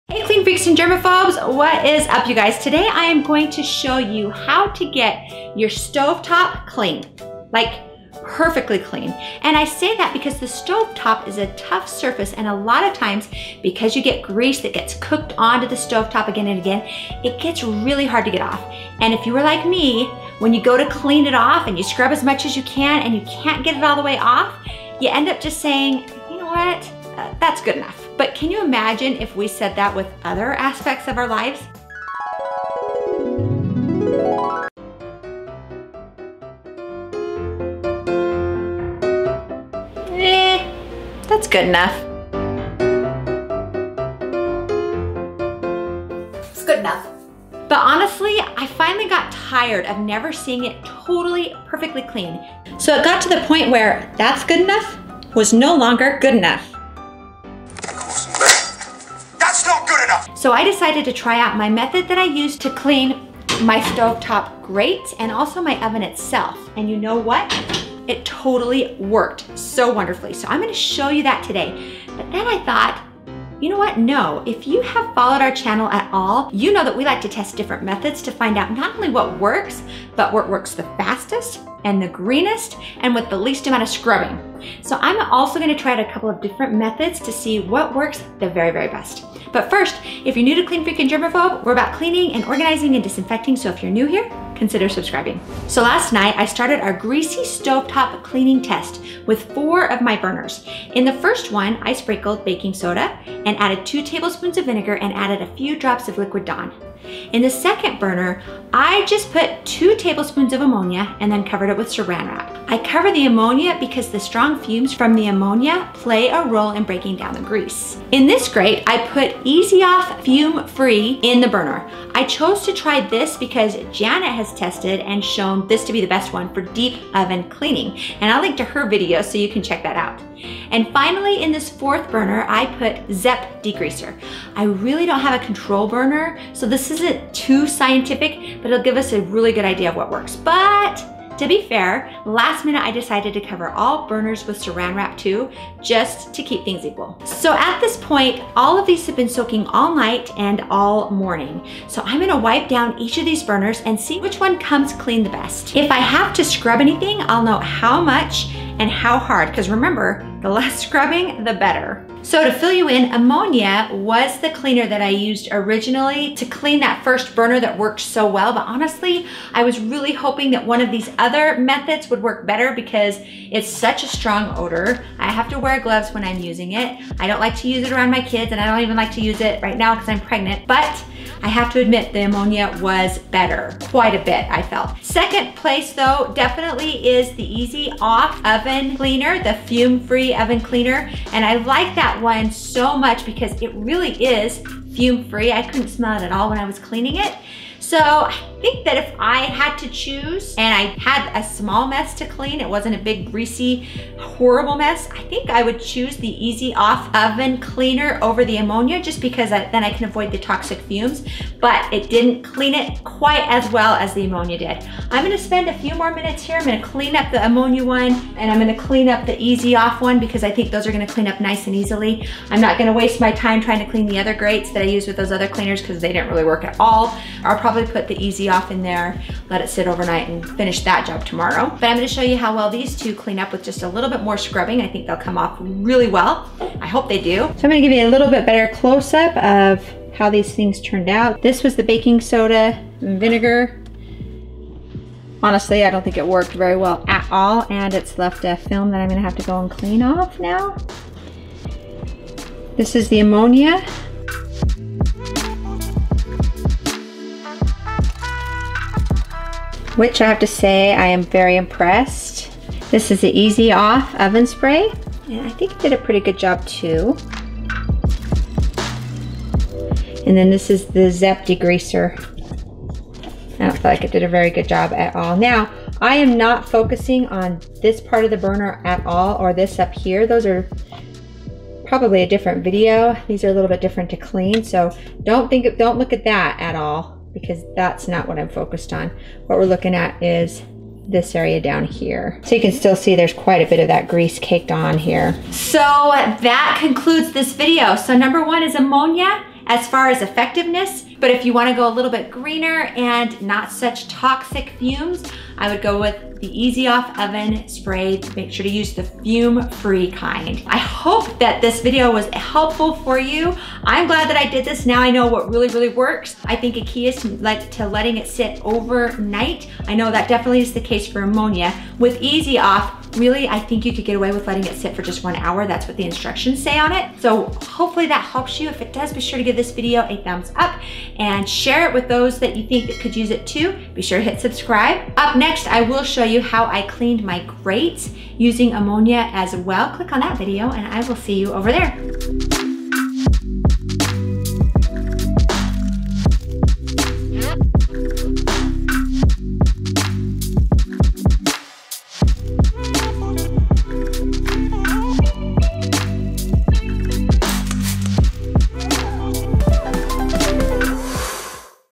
Easiest Way to Make the Stove Top Shine: Tested!
The easiest way to make the stove top shine.  Getting the stove top perfectly clean is a challenge.  There always seems to be a little burnt on grease that refuses to come up... until now.  I'm going to show you how to get it all perfectly clean.  Best part is, it requires the least amount of scrubbing too.

➡️ Zep Degreaser
➡️ Easy off Fume free oven spray

This is a more natural way to clean your gas stove top. This is a cleaning challenge. One method got it SO CLEAN with the least amount of work.

I tested several cleaning products: baking soda, vinegar and Dawn, Ammonia, Easy Off Fume Free oven cleaner, and Zep degreaser to clean the burners.